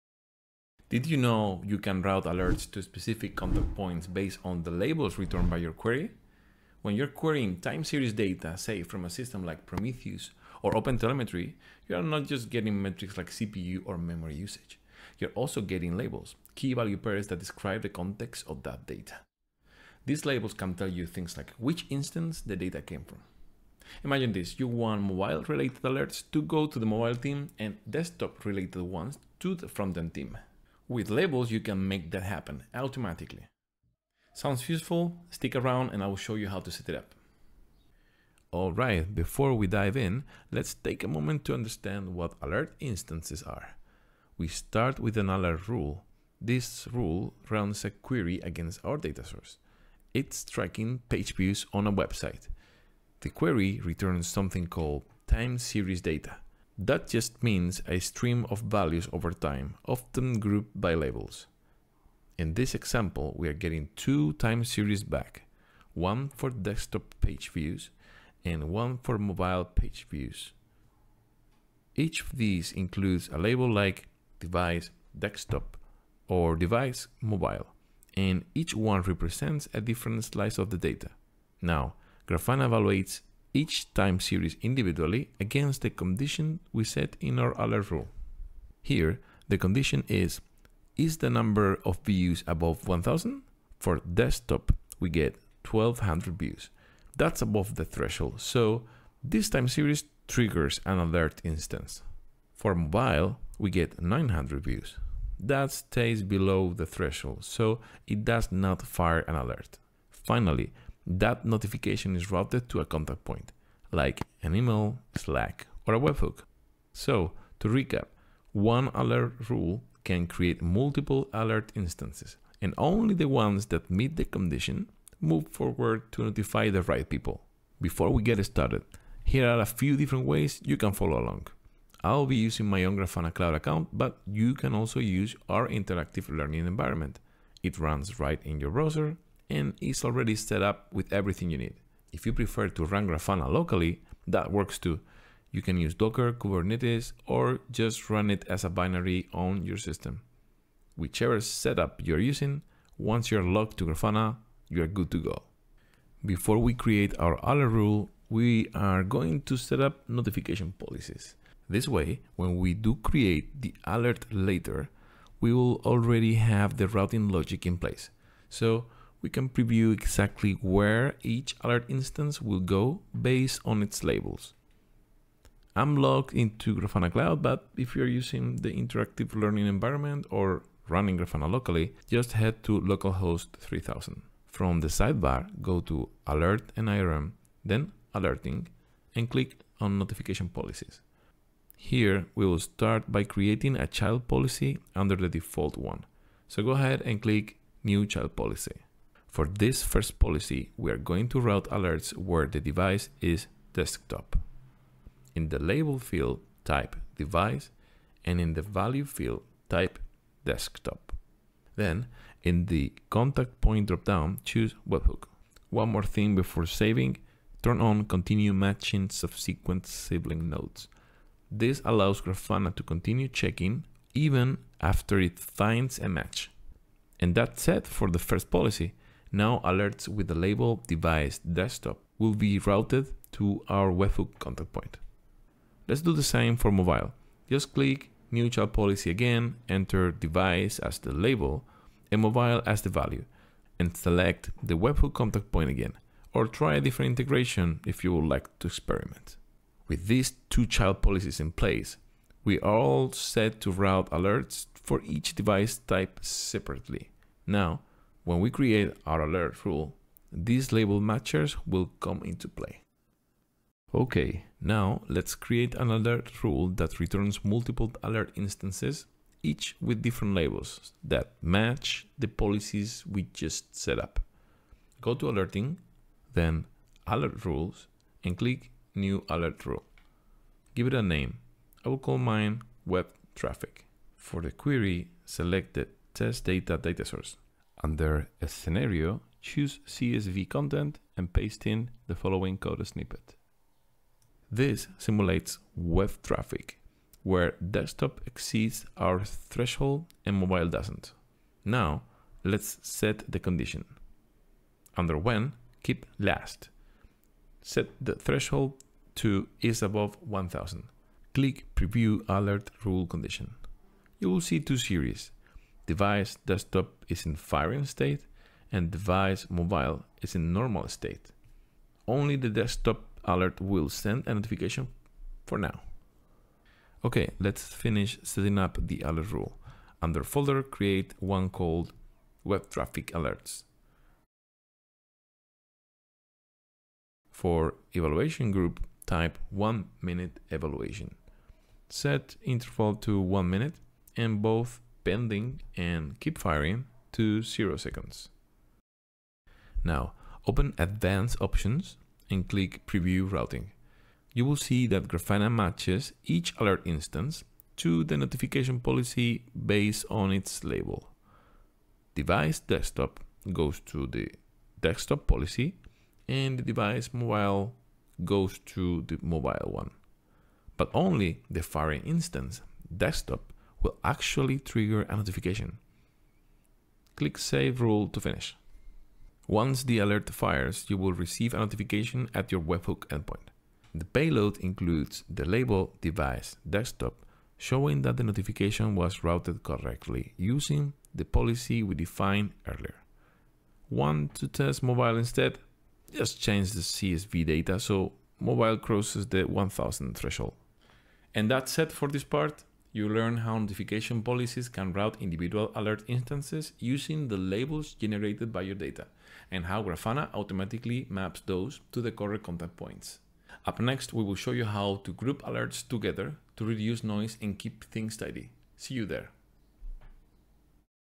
Get started with Grafana Alerting: Multi-dimensional alerts and how to route them
In this tutorial, we dig into more complex yet equally fundamental elements of Grafana Alerting: alert instances and notification policies.

Don't miss the rest of the "Get started with Grafana Alerting" series! Each part dives into a different feature to help you get the most out of alerting in Grafana.

🎥 Check out the full playlist here:
- Create and receive your first alert

TIMESTAMP
0:00 - Intro
0:37 - Alert instances
2:07 - Before you begin
2:35 - Notification policy setup
4:24 - Create an alert rule
6:18 - Receive alert notifications
6:40 - Conclusion

❓ Have a question that isn't related to this video?